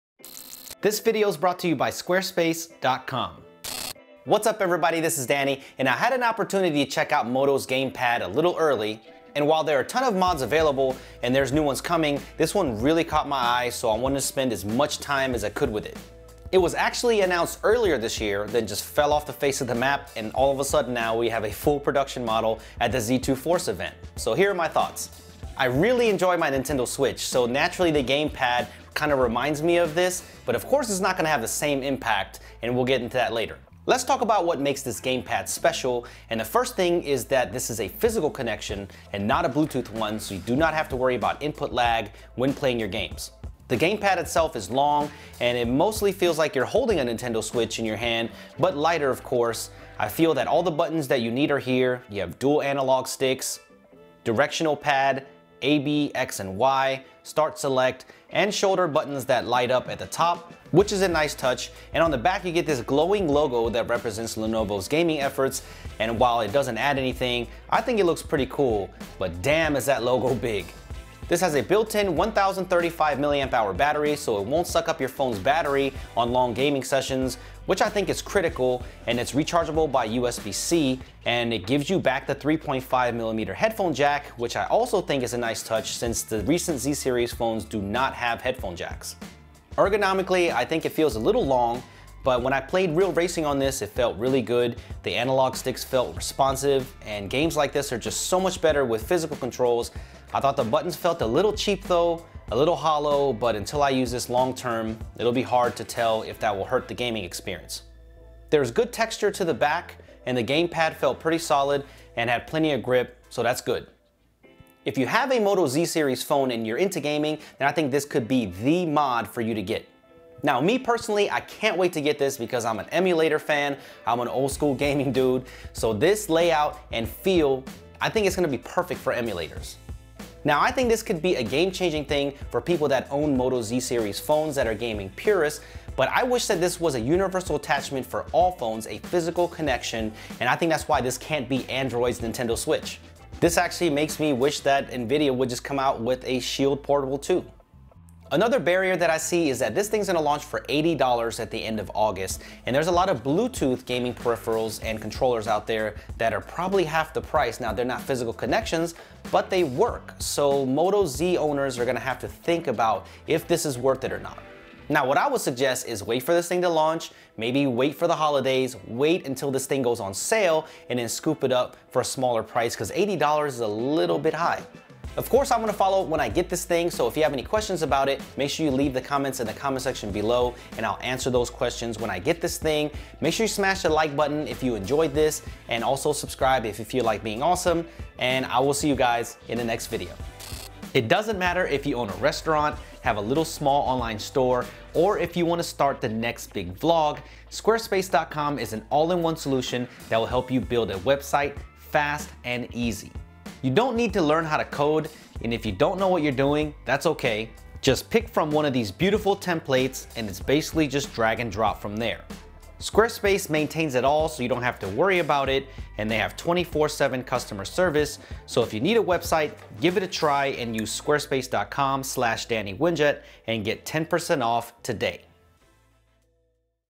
Moto Gamepad Hands On: Android's Nintendo Switch?
I got to check out Moto's gamepad for the moto z2 force and other moto z line up of smartphones.  Android's Nintendo Switch?

Built in battery, 3.5 mm headphone jack and more! Can this compete with the Nintendo Switch? No.  But it can make your games better on Android. Full moto gamepad review soon!